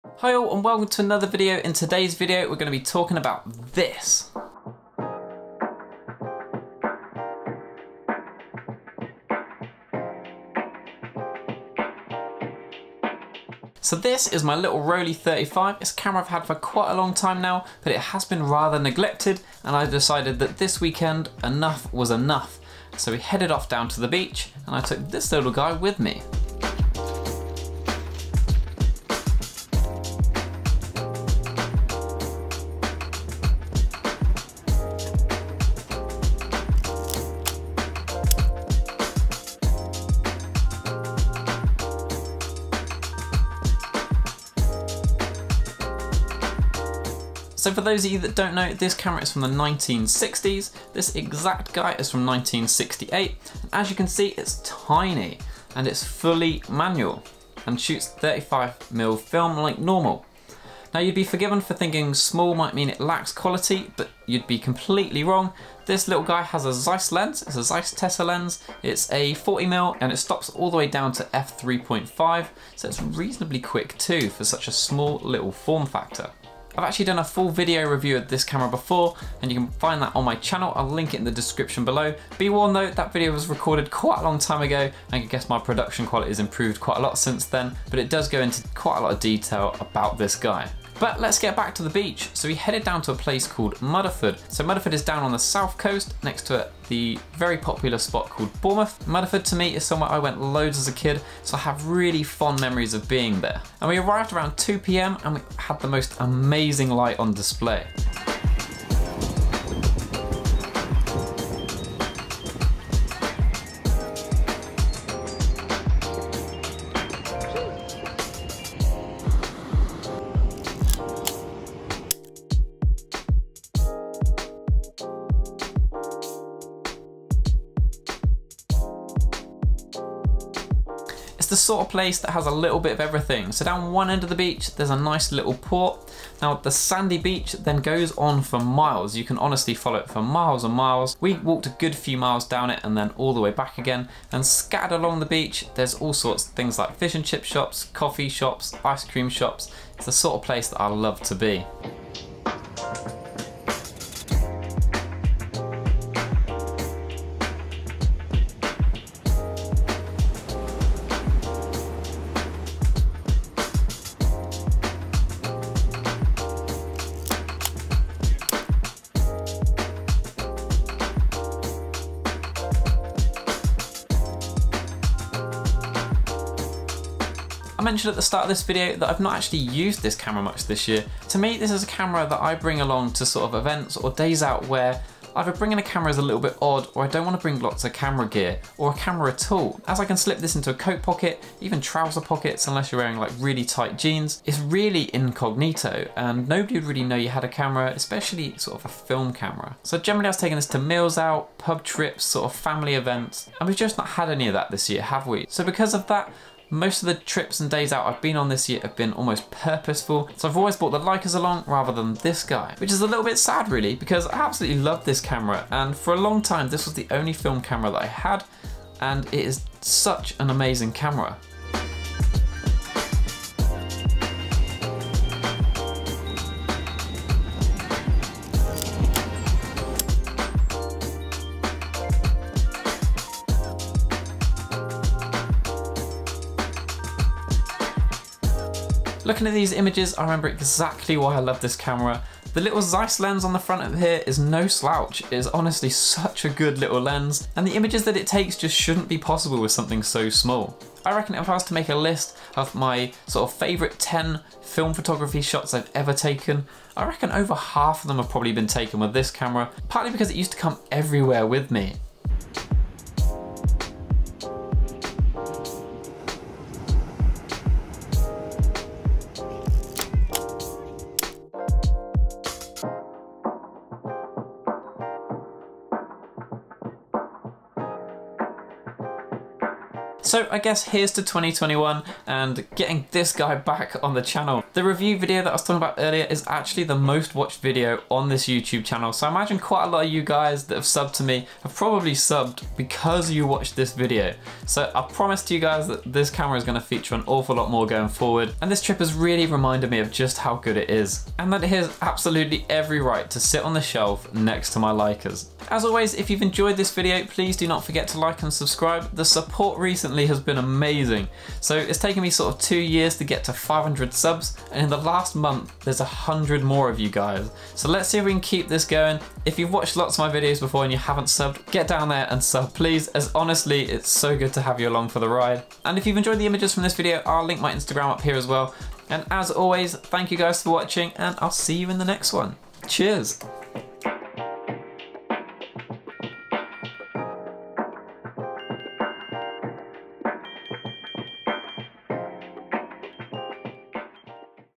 Whats happened to my Rollei 35? - Mudeford Beach
Hi All,

And welcome to another video. In todays video we talk about the Rollei 35. Which is an amazing little 35mm film camera from the 1960s. Theres loads of versions of this camera and they are all so so so good!

I haven't used this camera much this year and in todays view we discuss why that is

Any questions about the camera, give me a shout in the comments below